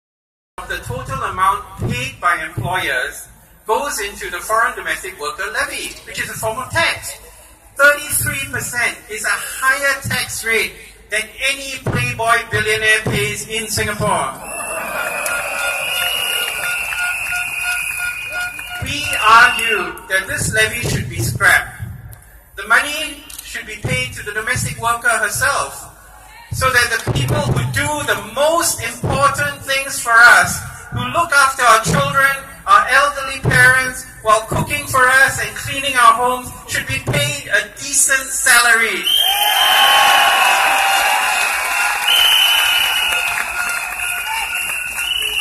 SDP's Paul Tambyah on maid levy
He says the 33% levy is too high, and should be paid to the maids, who do so much work for us. Video shot by Calvin Yang on Sept 9, 2015.